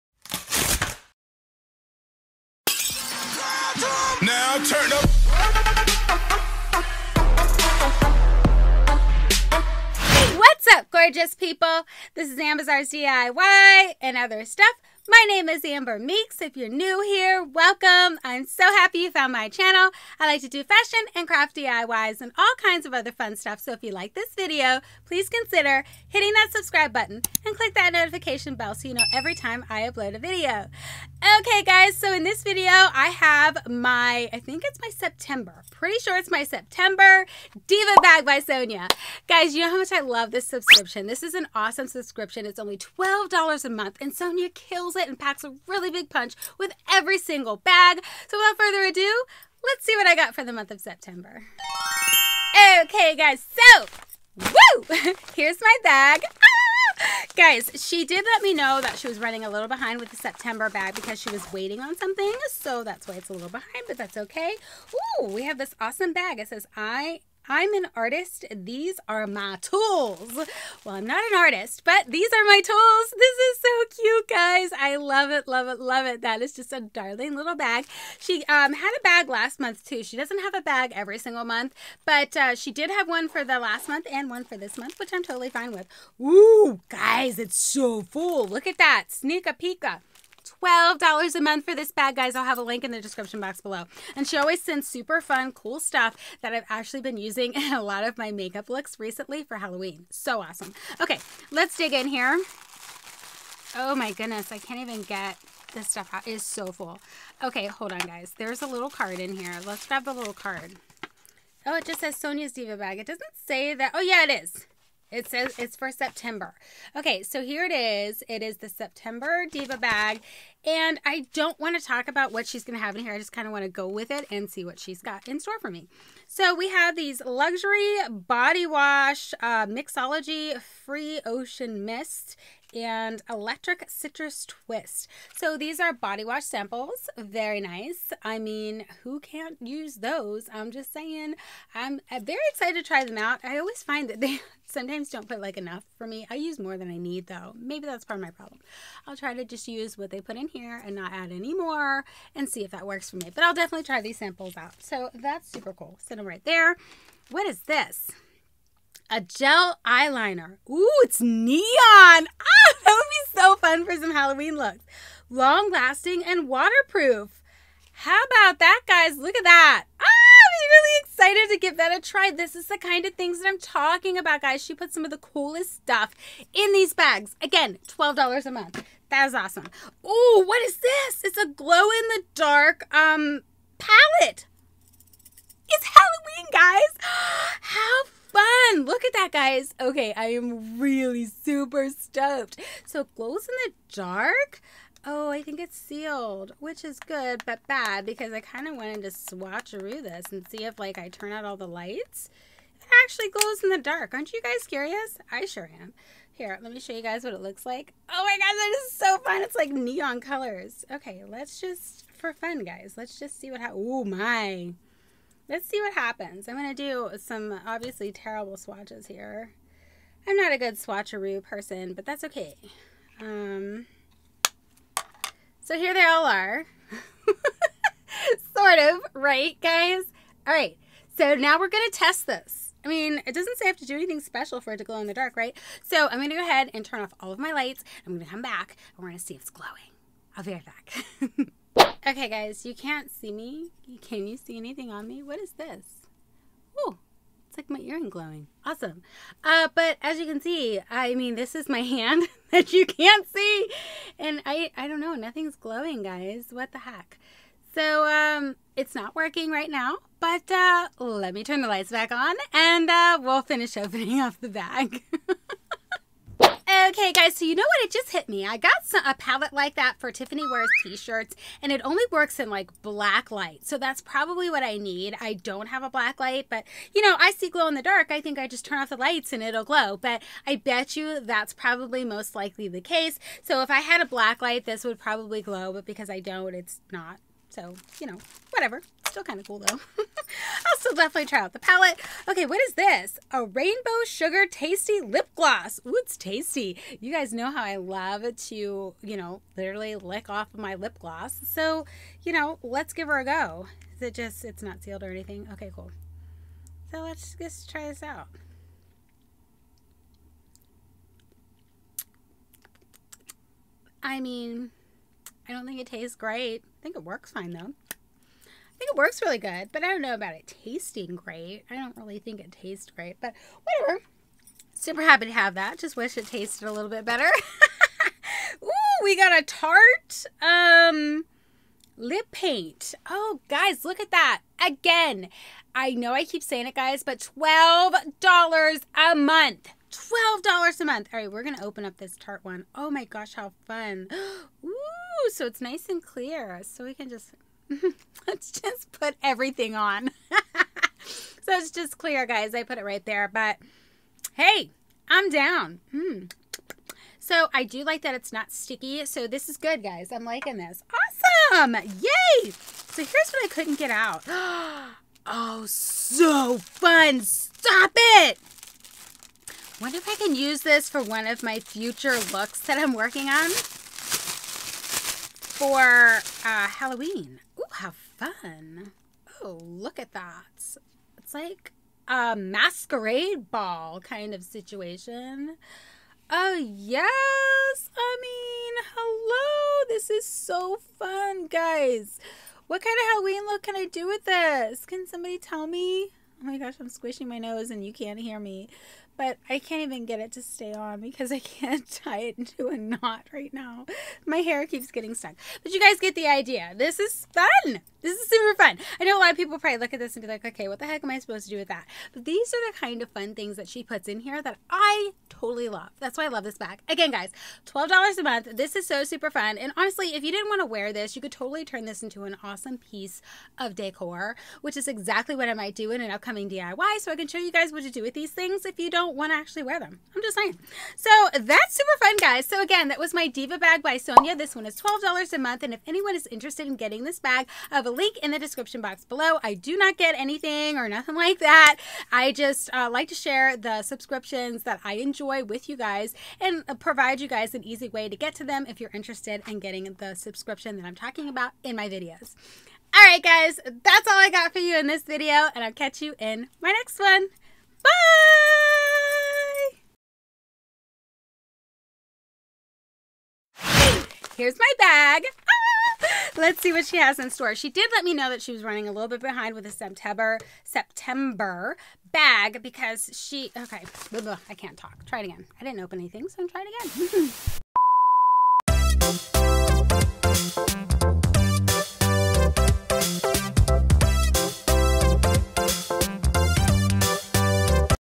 September 2021 - Diva Bag by Sonya! Glow-in-the-Dark Eyeshadow Cool!
What's up Gorgeous People?!?!

For today's video, I have for you my September 2021 Diva Bag by Sonya. This beauty bag is only $12 a month and packs a pretty BIG punch for that amount of money. I am very curious about the glow-in-the-dark eyeshadow she sent this month.  Sonya does a fabulous job with this Diva Bag and I look forward to it every month. I hope you enjoy the video.

0:00 Intro
0:52 Unboxing
7:10 Final Thoughts
7:39 Outtakes

TikTok ambizzares-amber meeks

Amber Meeks
Imperial, PA 15126